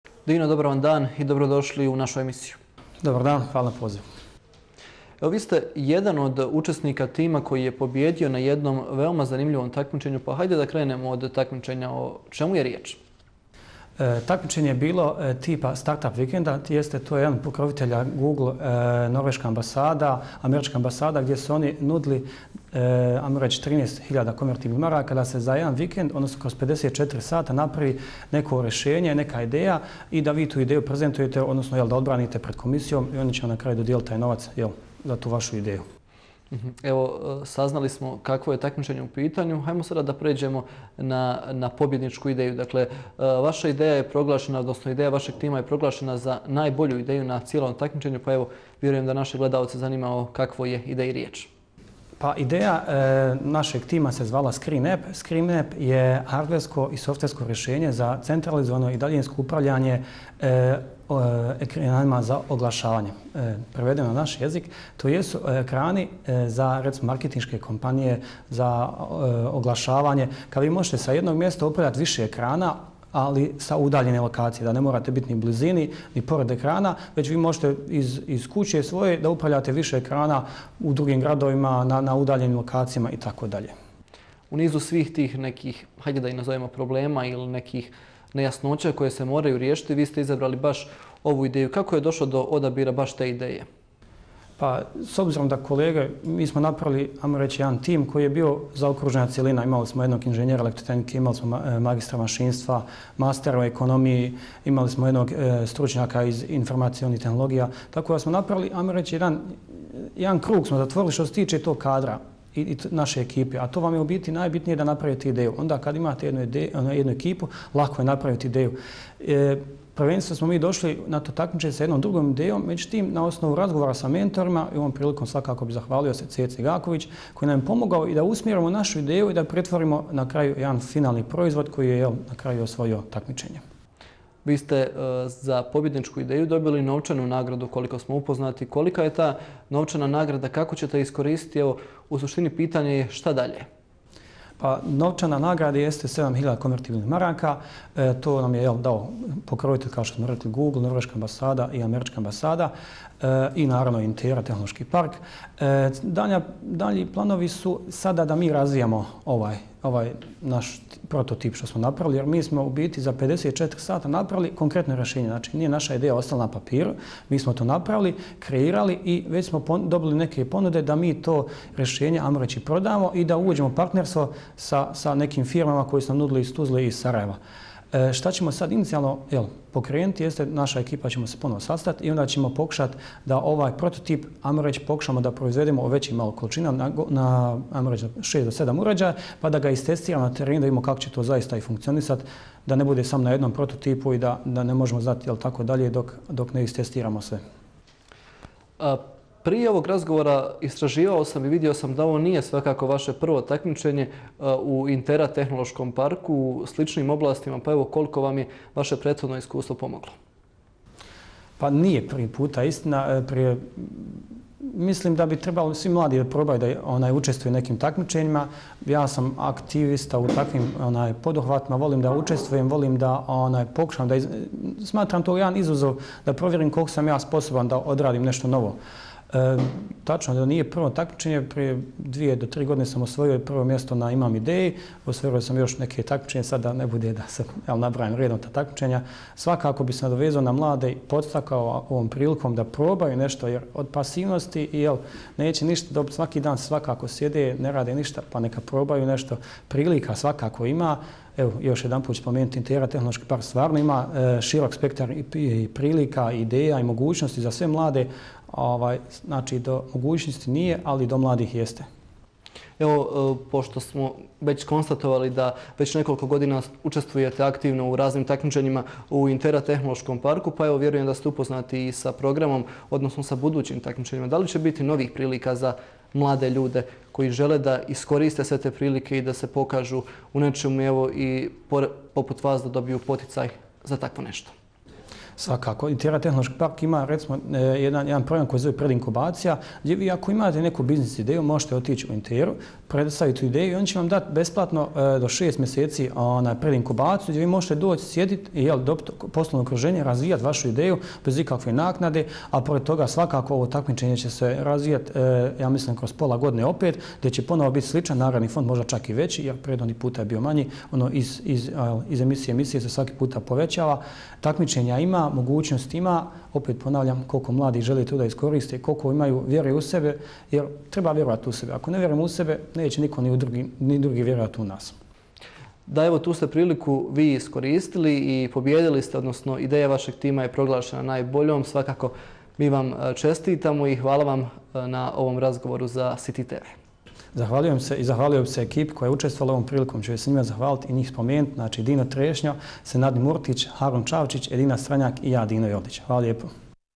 Intervju s Dinom Joldićem
Dino Joldić je član Screen App pobjedničkog tima proljetnog Startup Weekend-a održanog u Intera tehnološkom parku. Pogledajte za kakvu poslovnu ideju su dobili novčanu nagradu, da li će uslijediti implementacija ideje, a doznali smo i odgovor na pitanje ima li dovoljno poticaja za mlade i kreativne ljude u našem gradu.